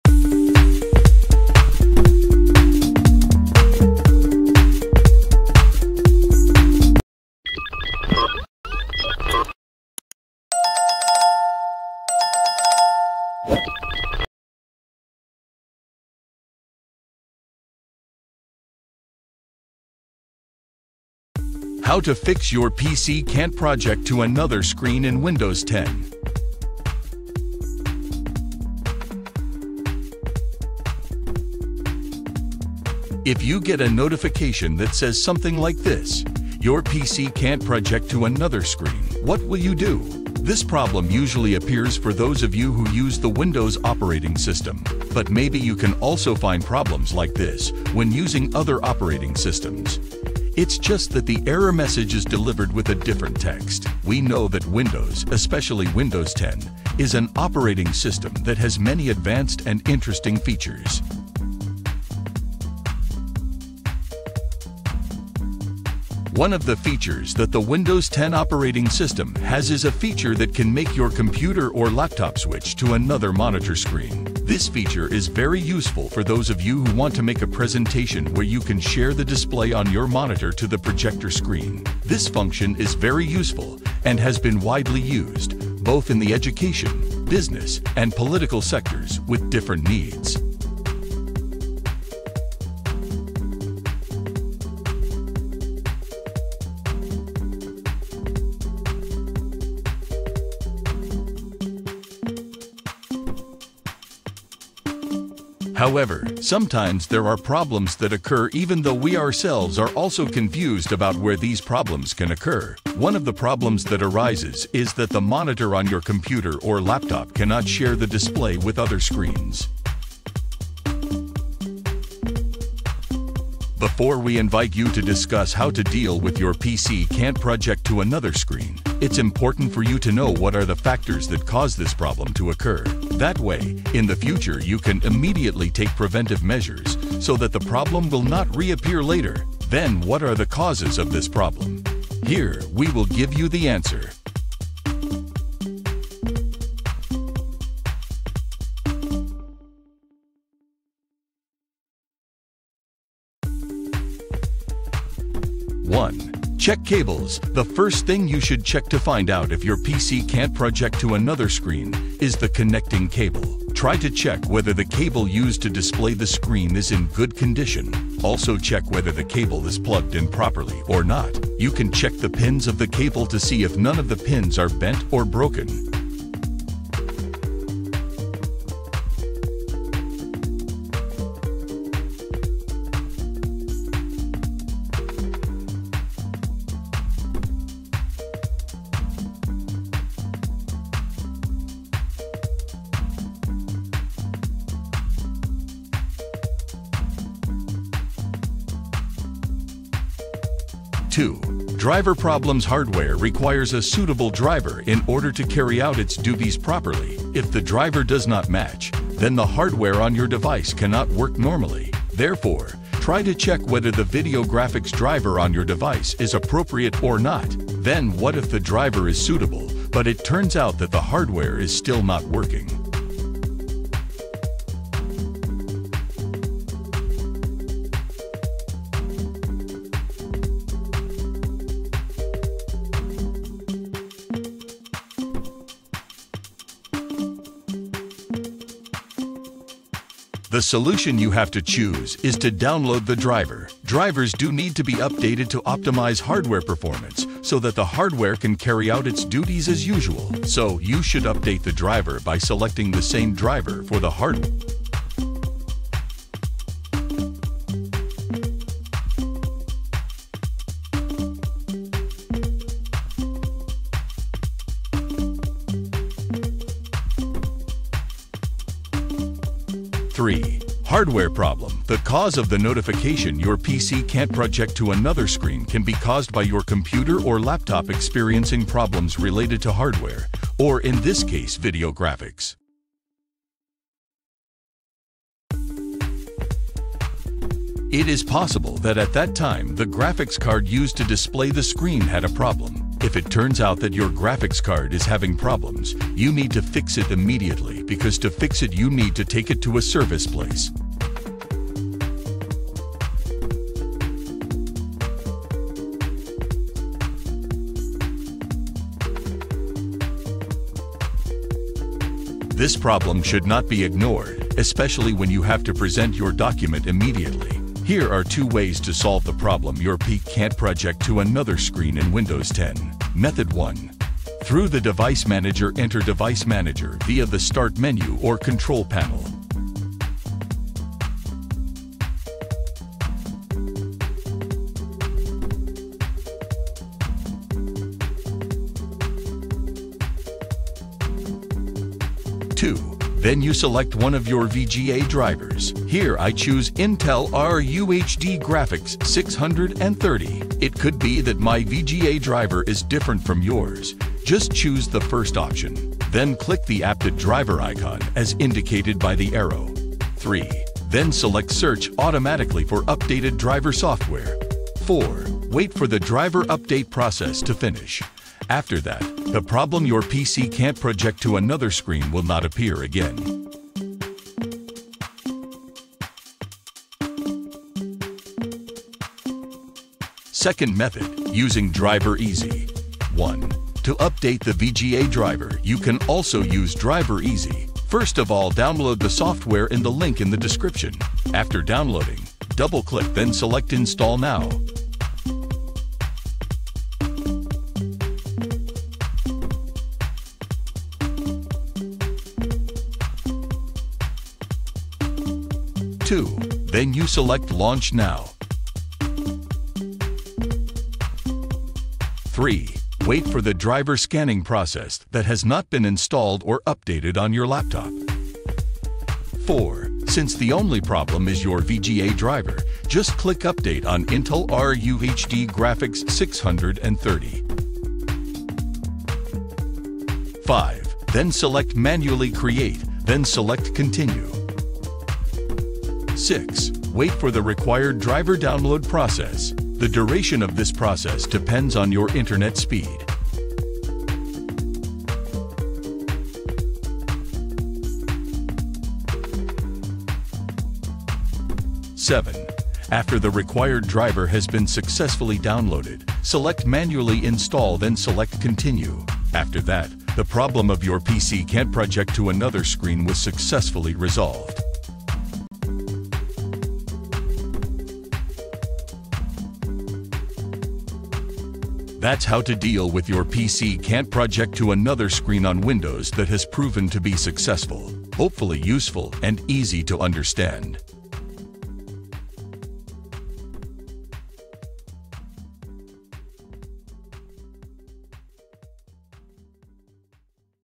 How to Fix : Your PC can't project to another screen Problem - on Windows 10
Your PC can't project to another screen, what will you do?
This problem usually appears for those of you who use the Windows operating system. But maybe you can also find problems like this when using other operating systems.

It's just that the error message is delivered with a different text. We know that Windows, especially Windows 10, is an operating system that has many advanced and interesting features.